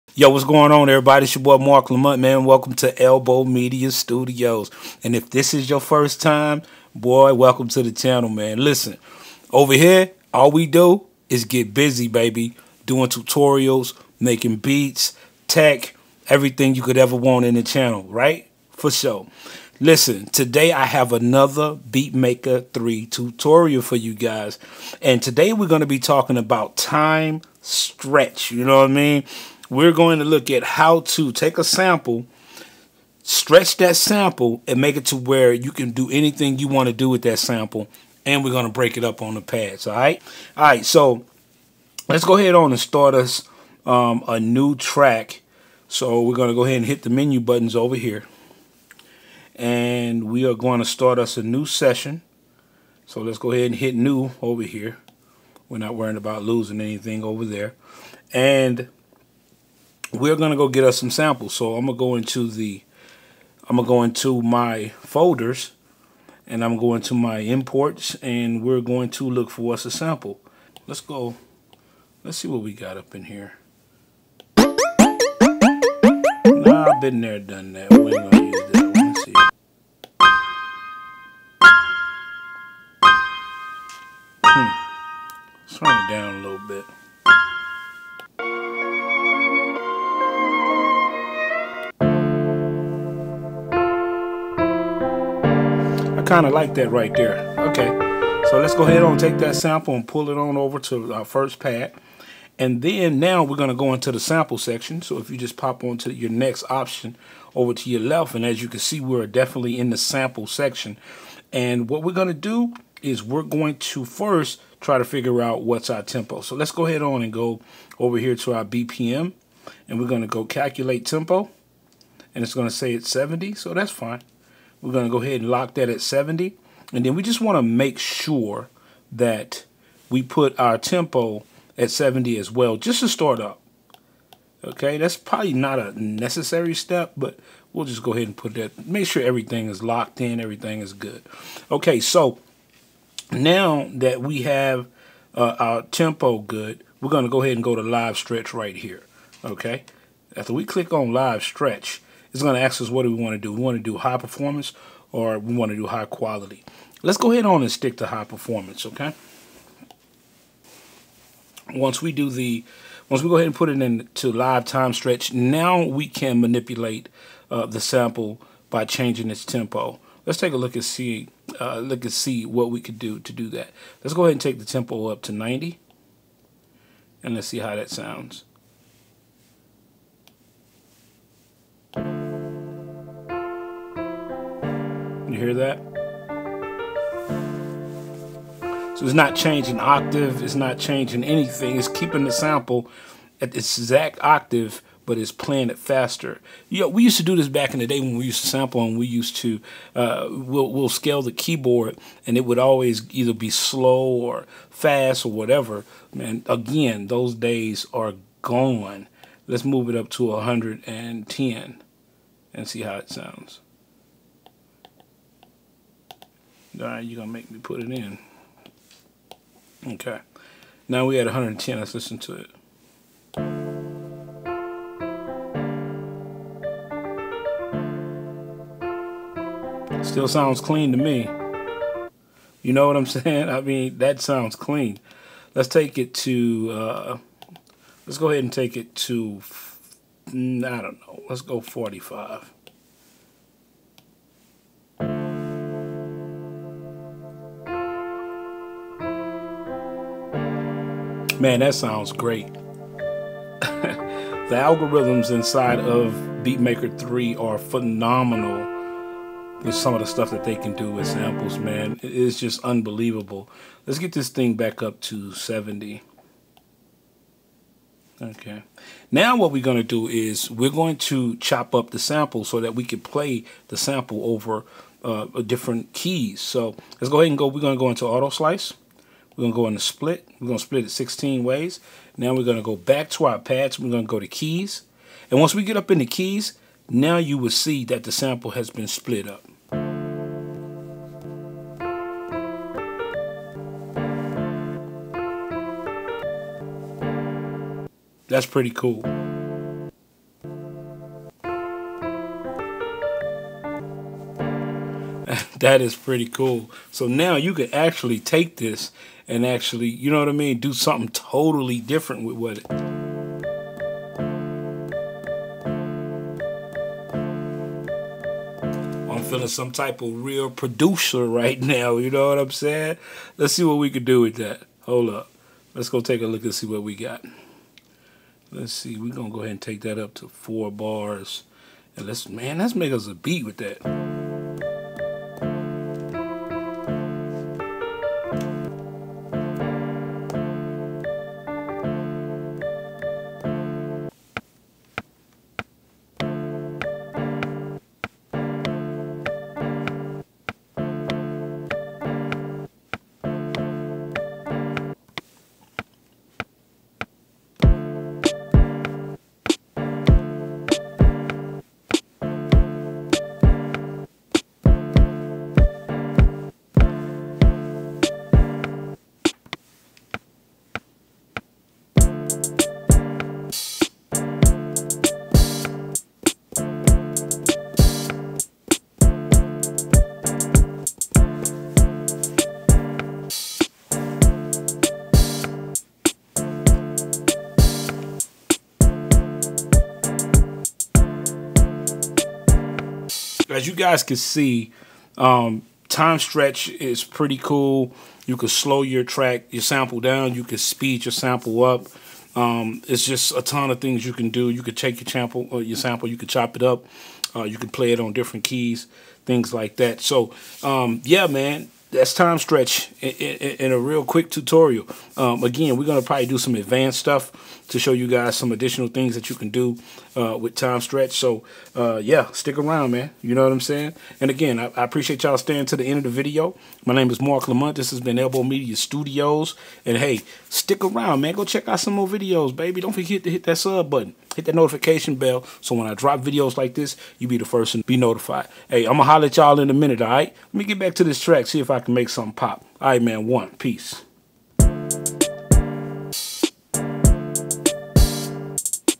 BeatMaker 3 Tutorial | How to Time Stretch Samples
What's Good YouTube Family! We are back again with another BeatMaker 3 tutorial! This time...we are going to show you how to Time Stretch a sample in BeatMaker 3. This should be fun!! Enjoy!